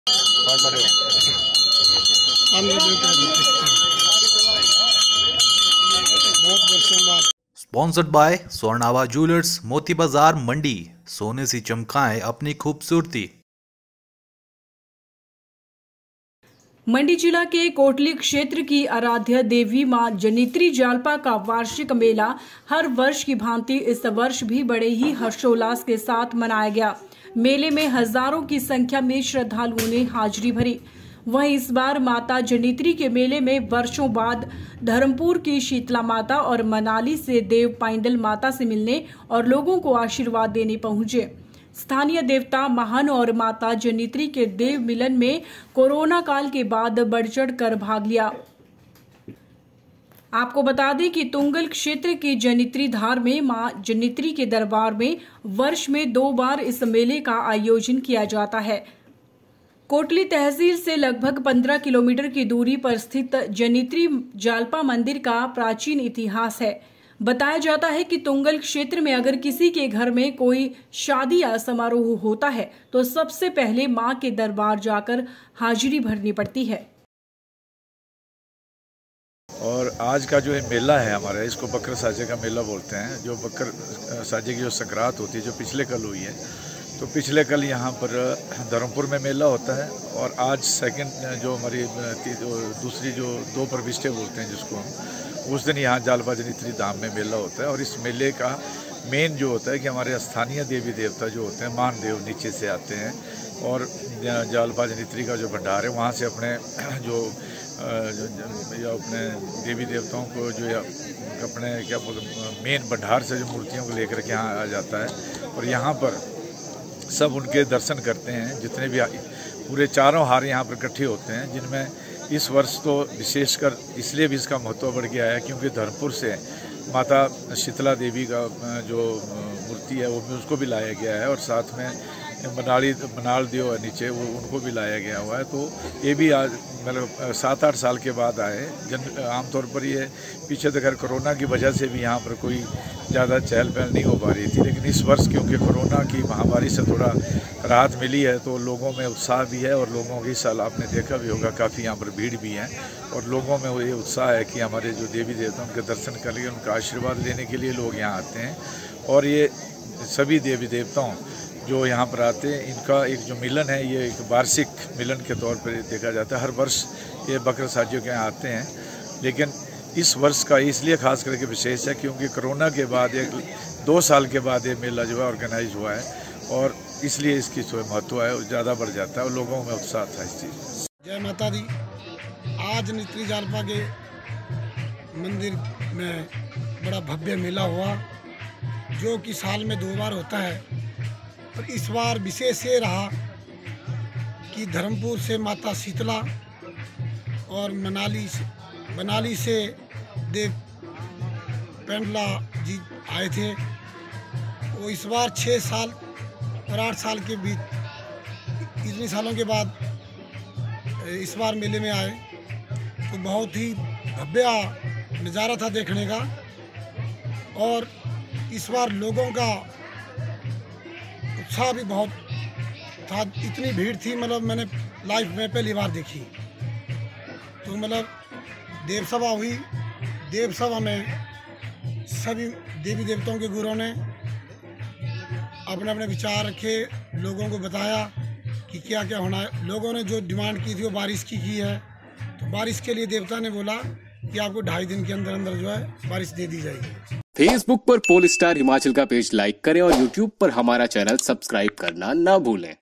हर्षोल्लास के साथ मनाया गया माता जनितरी जालपा मेला।
मंडी जिला के कोटली क्षेत्र की आराध्या देवी मां जनितरी जालपा का वार्षिक मेला हर वर्ष की भांति इस वर्ष भी बड़े ही हर्षाेल्लास के साथ मनाया गया। मेले में हज़ारों की संख्या में श्रद्धालुओं ने हाज़री भरी। वहीं इस बार माता जनितरी के मेले में वर्षों बात धर्मपुर की शीतला माता और मनाली से देव पाइंदल माता से मिलने व लोगों को आशिर्वाद देने पहुंचे।